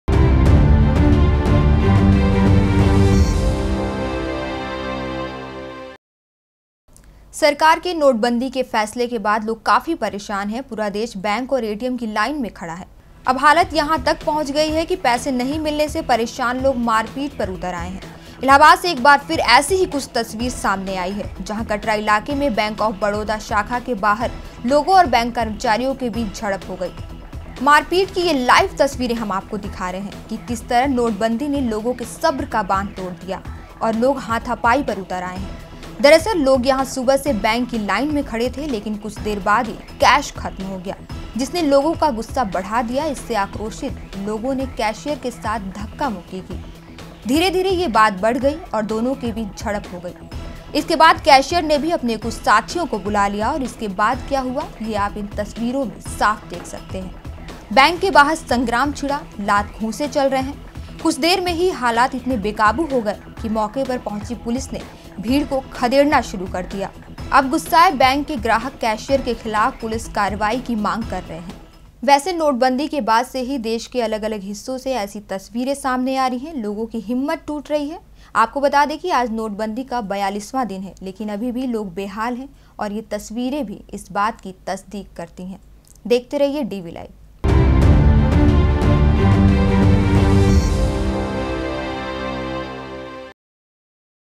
DB LIVE | 21 DEC 2016 | People grow impatient waiting for cash at banks
सरकार के नोटबंदी के फैसले के बाद लोग काफी परेशान है. पूरा देश बैंकों और एटीएम की लाइन में खड़ा है. अब हालत यहां तक पहुंच गए है कि पैसे नहीं मिलने से परेशान लोग मारपीट पर उतर आएं हैं. इलाहाबाद से एक बार फिर ऐसी ही कुछ तस्वीरें सामने आई हैं..जहां कटरा इलाके में बैंक ऑफ बड़ौदा शाखा के बाहर लोगों और बैंक कर्मचारियों के बीच झड़प हो गई...मारपीट की ये LIVE  तस्वीरें हम आपको दिखा रहे हैं...कि किस तरह नोटबंदी ने लोगों के सब्र का बांध तोड़तोड़ दिया और लोग हाथापाई पर उतर आए हैं...दरअसल लोग यहां सुबह से बैंक की लाइन में खड़े थे...लेकिन कुछ देर बाद ही कैश खत्म हो गया ..जिसने लोगों का गुस्सा बढ़ा दिया...इससे आक्रोशित लोगों ने कैशियर के साथ धक्का मुक्की की...धीरे धीरे ये बात बढ़ गई..और दोनों के बीच झड़प हो गई...इसके बाद कैशियर ने भी अपने कुछ साथियों को बुला लिया.,.और इसके बाद क्या हुआ...ये आप इन तस्वीरें में साफ देख सकते है...बैंक के बाहर संग्राम छिड़ा...लात-घुसे चल रहे है..कुछ देर में ही हालात इतने बेकाबू हो गए कि मौके पर पहुंची पुलिस ने भीड़ को खदेड़ना पड़ा... अब गुस्साए बैंक के ग्राहक कैशियर के खिलाफ पुलिस कार्रवाई की मांग कर रहे हैं